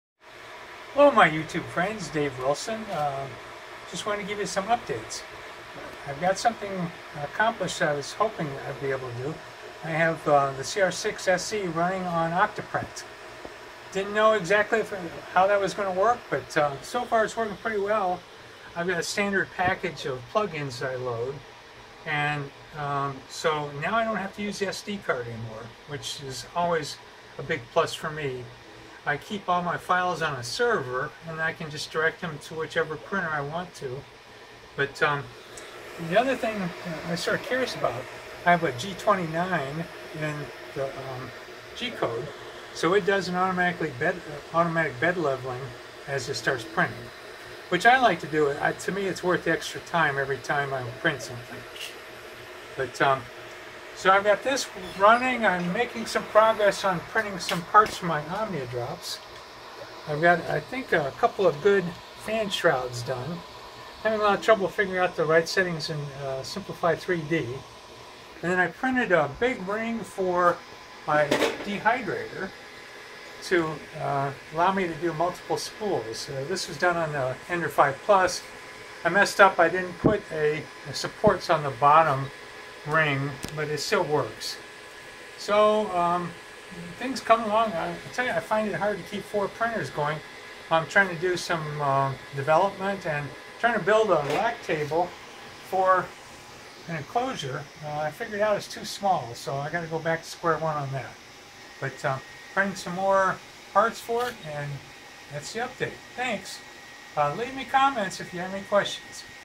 CR-6 SE updates
I wanted to get some information out about the Creality CR-6 SE, since a lot of people are asking questions.   I was able to get this running on OctoPrint with no problem.  I do things a little differently, I have a separate Pi for each printer.  For me this makes it easier, and I change the host name to identify the printer.  This way I can call the printers by name in my browser and not worry about the IP address.  I also run all of my OctoPrints using wired Ethernet, since I'm next to my 24 port switch and it makes it more reliable..